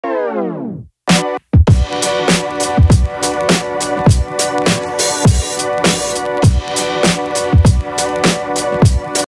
NY Fisher CAT
New York state Fisher that was making noise and running around all morning.